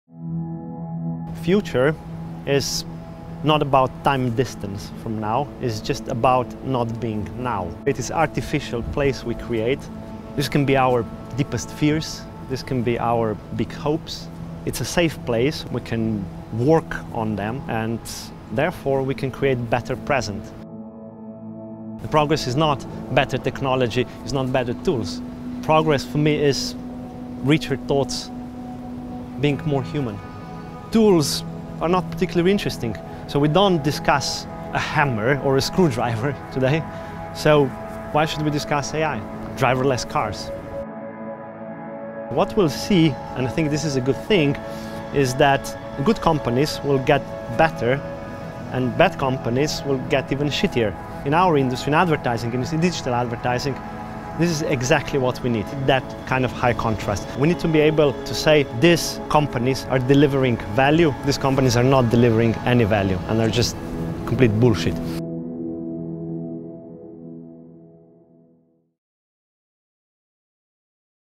Matevz Klanjsek | Create The Future 3 of 4
Matevz Klanjsek, Co-Founder & Chief Product Officer at Celtra, talks to us ahead of his appearance at IAB Engage: Create The Future.

When is the future? 0.00
What future would you like to see? 0.20
What should our future be? 0.42